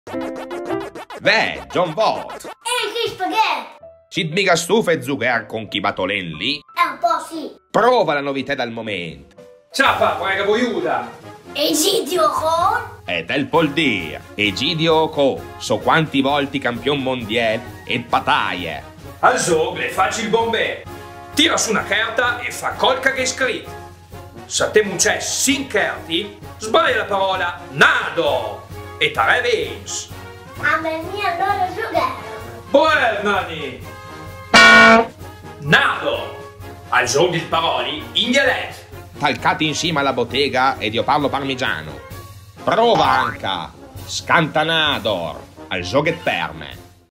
Nàdor - il gioco delle parole in dialetto
Al zógh dil paròli in djalètt!

La regola è solo una: scegli una carta e segui le istruzioni. Il primo che ne conquista 5, vince.

Game Design - Simone Terenziani di Comelasfoglia Studios
Artwork - Miguel Guercio
Dialetto e Fonetica - io parlo parmigiano

Prova anche Scantanàdor!

Troverai entrambi i giochi ad un prezzo speciale al seguente

Prodotto da Demoela.